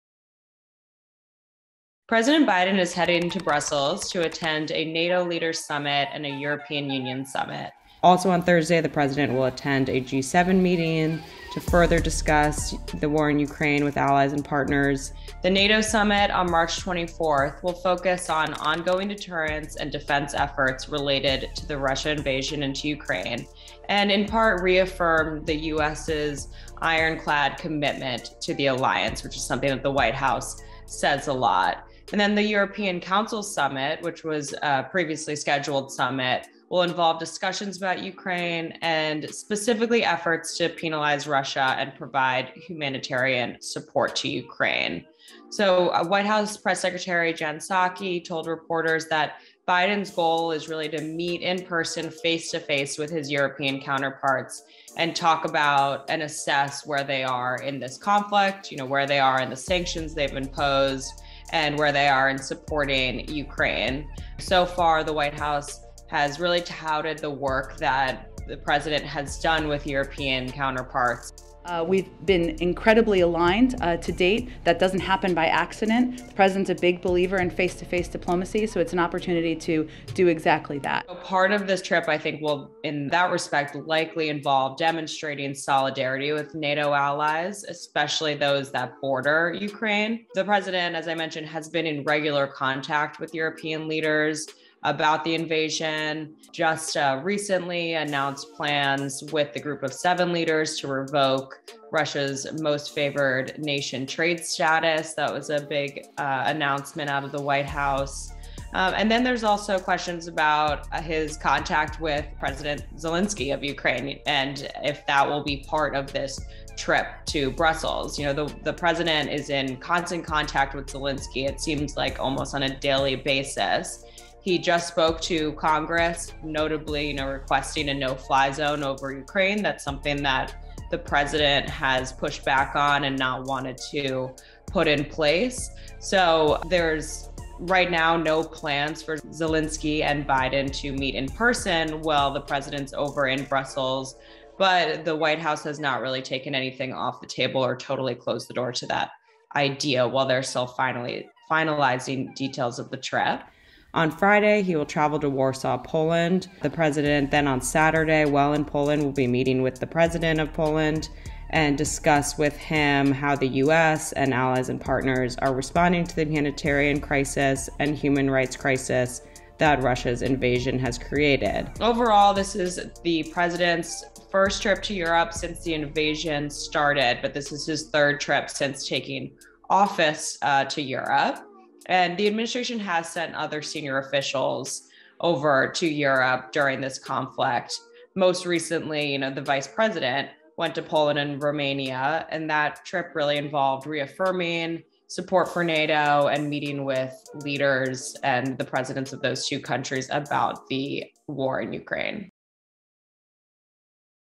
Here's President Biden's Schedule For His Critical Trip To Europe This Week
This week, President Biden is scheduled to make his first trip to Europe since Russia invaded Ukraine.